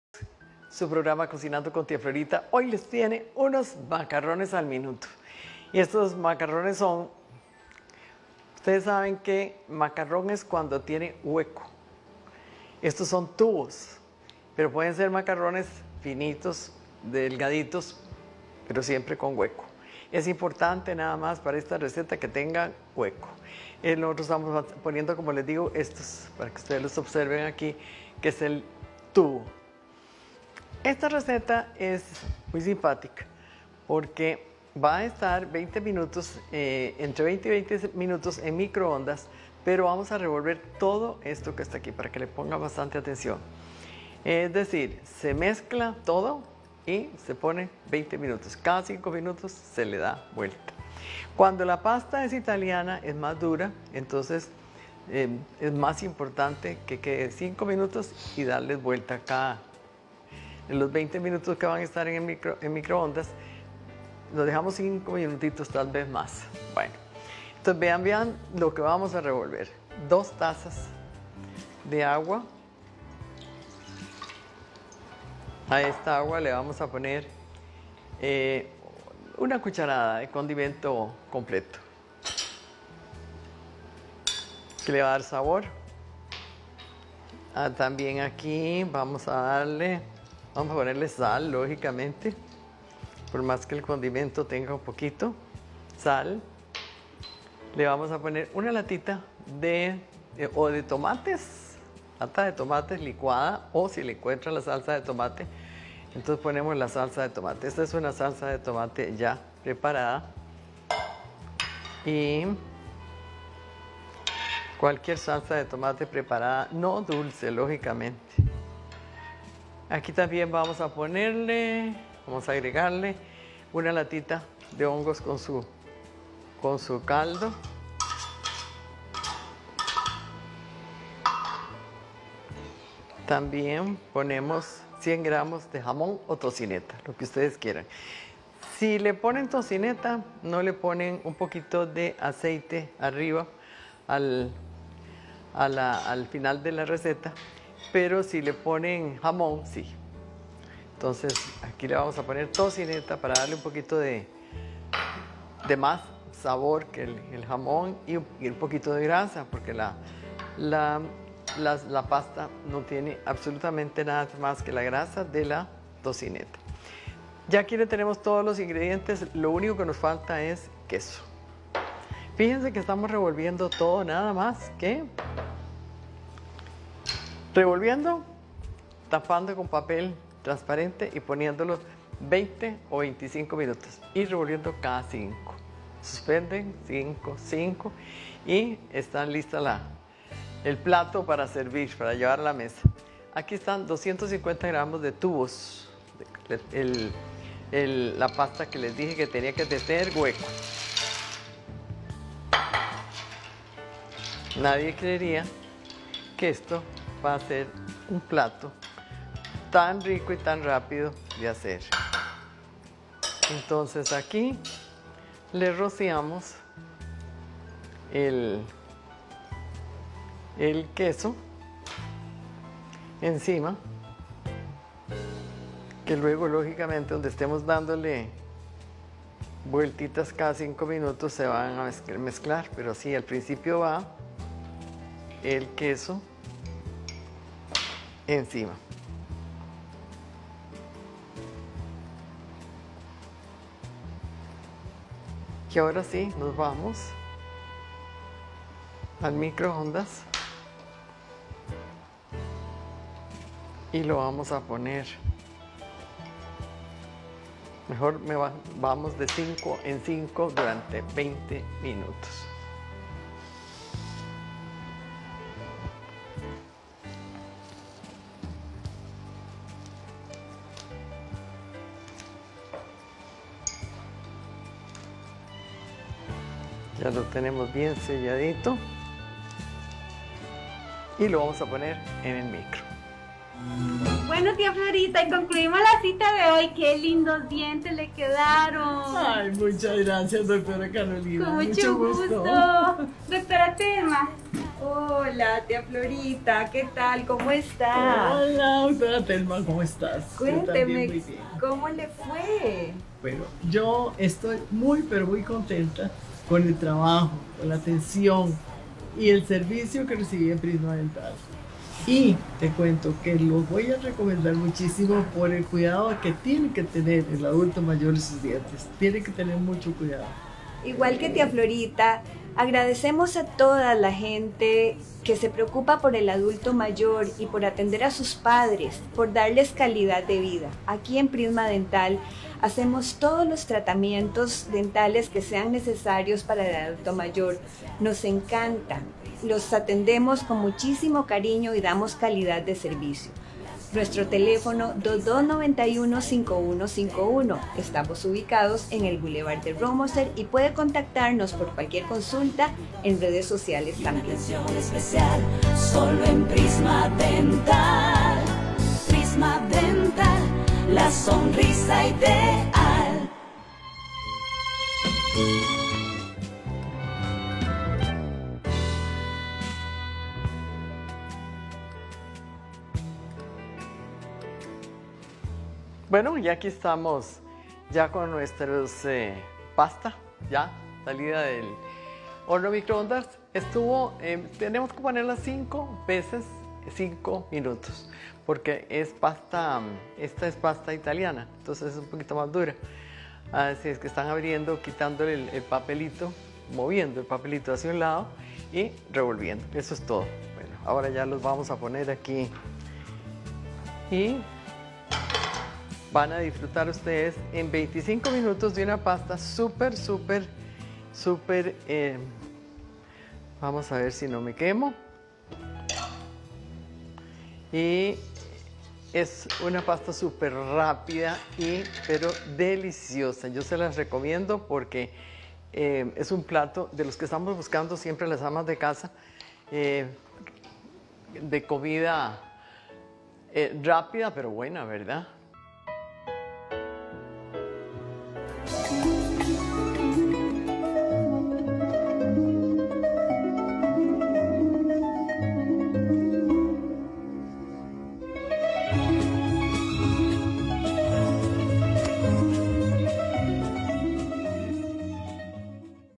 MACARRONES AL MINUTO
Ingredientes
250 gramos macarrones (pasta de hueco)
2 tazas agua
1 lata tomates preparada con albahaca, orégano y ajo
2 cucharadas aceite oliva
1 cucharada condimento completo
1 cucharadita sal
100 gramos tocineta o jamón picado o salchicha, etc.
1/2 taza queso parmesano

Licuar tomates de lata
Partir pasta en tres.
Poner todos los ingredientes en un refractario grande y hondo (u otro grande de cristal) y revolver.

Poner en microondas en HIGH durante 20 minutos, revolviendo cada 5 minutos (cuatro veces). Nota: si se usa pasta Italiana más dura, tapar.
Sacar, rociar de queso parmesano y servir inmediatamente.
4 porciones